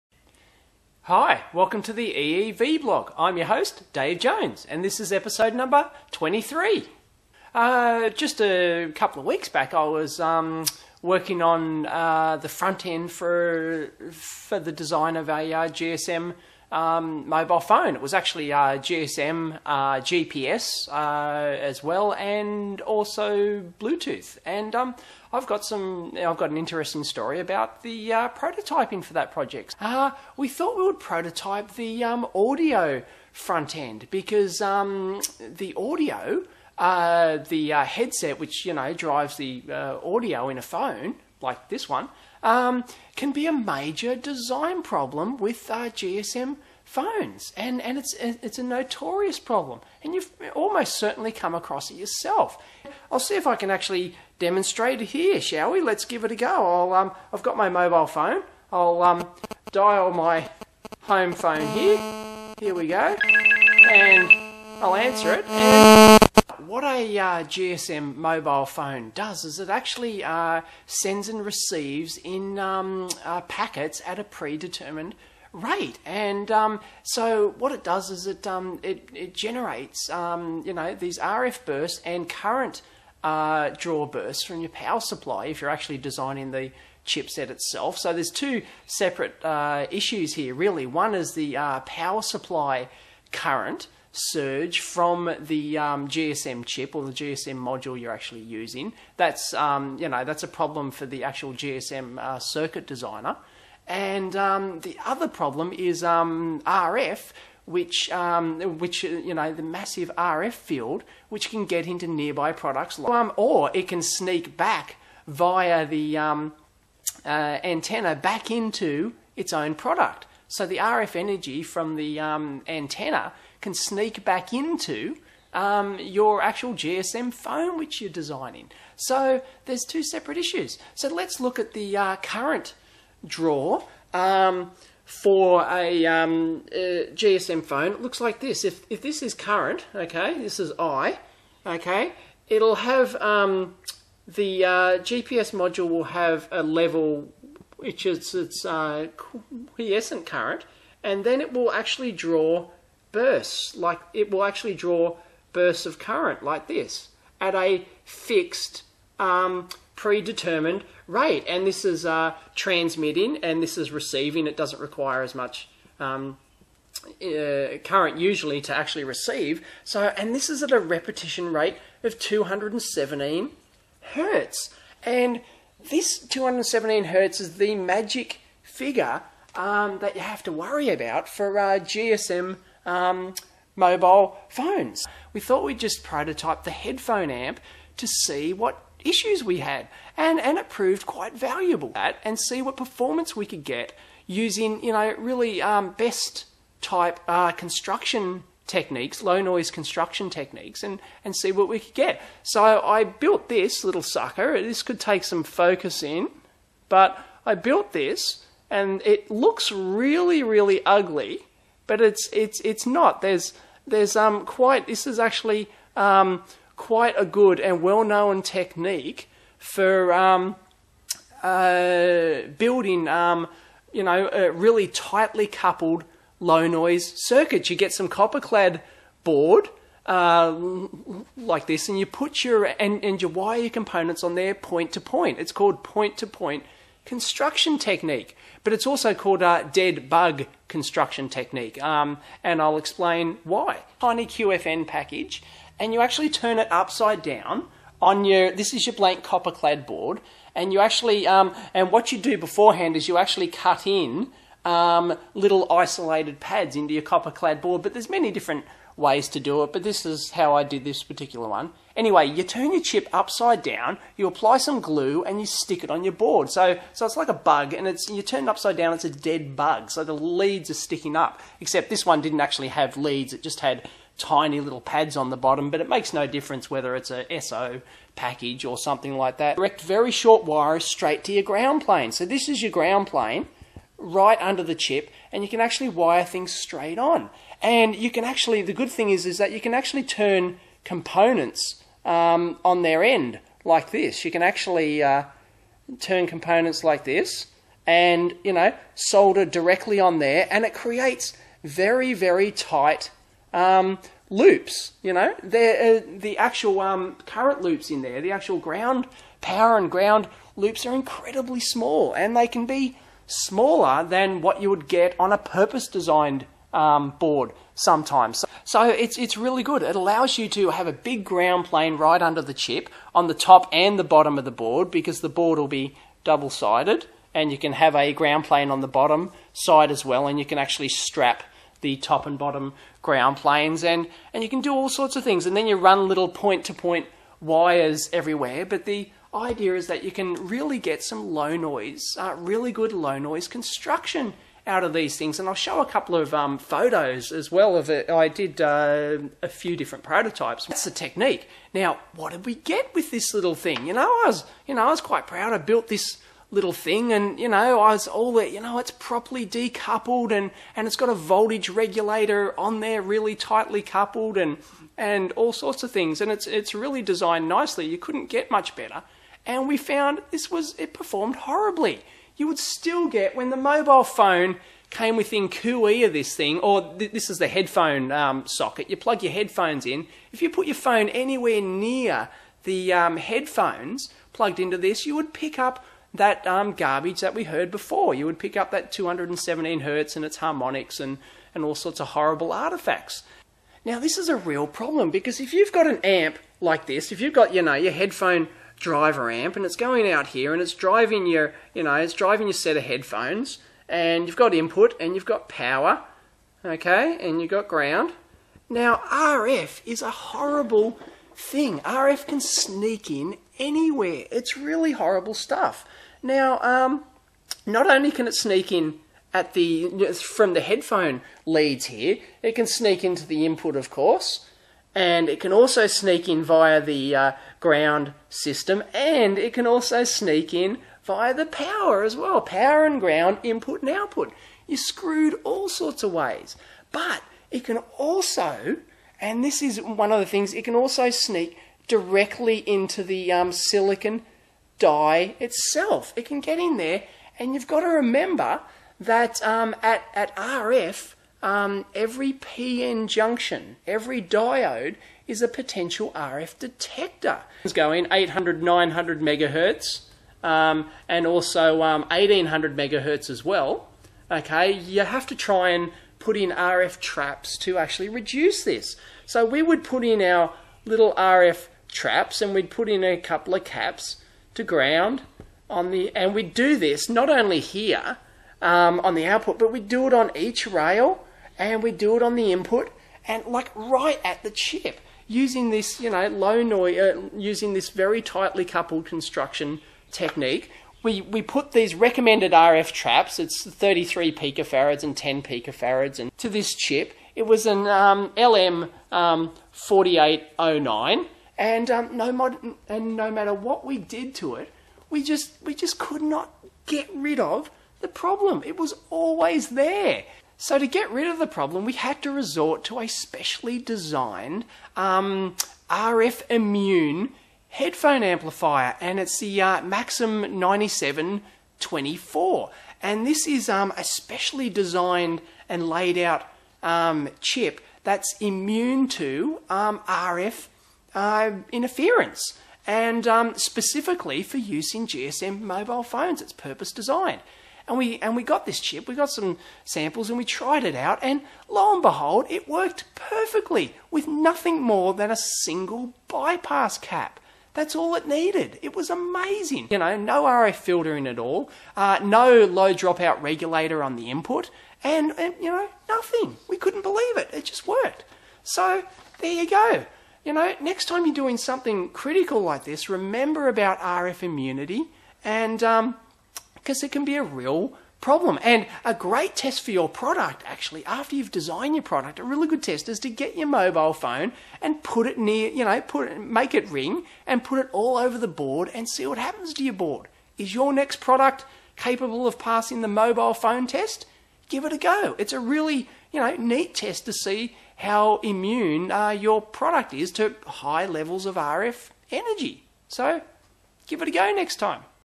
EEVblog #23 - GSM mobile phone audio design
GSM mobile phone audio design, RF immunity, and low noise dead bug point-to-point prototyping.